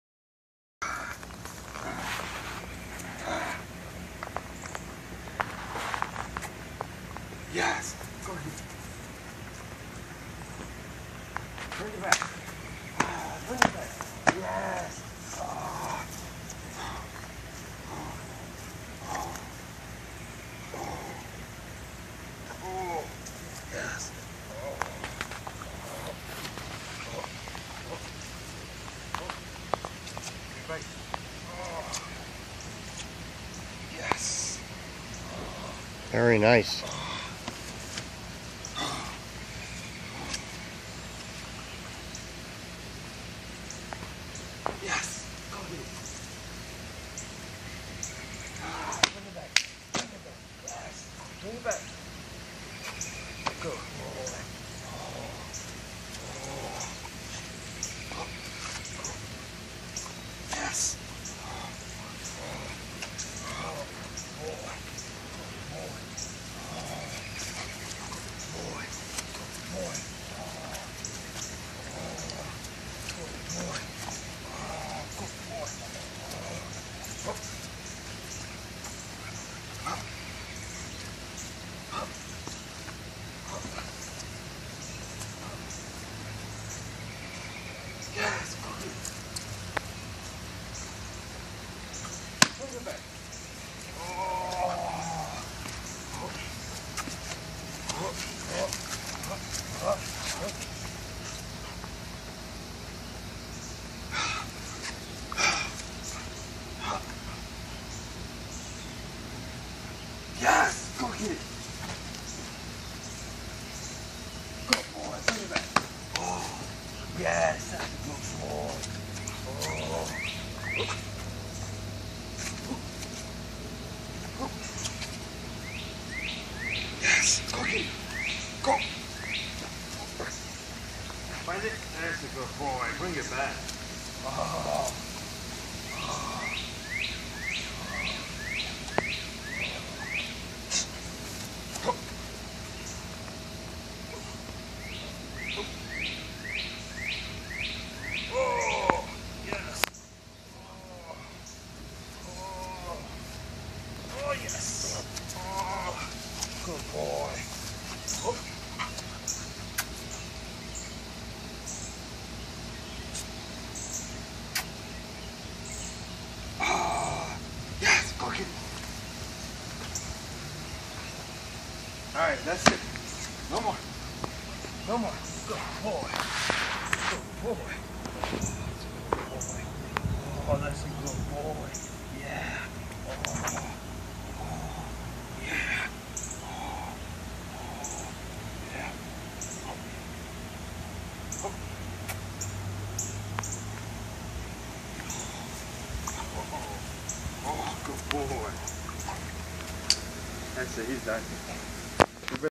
Building Ball drive with Reaper
Building ball drive with Reaper 5 month old Blue Seal male. Extremely SMART! Picked up on this in less yhen a minute. Can't wait till he's at his full potential.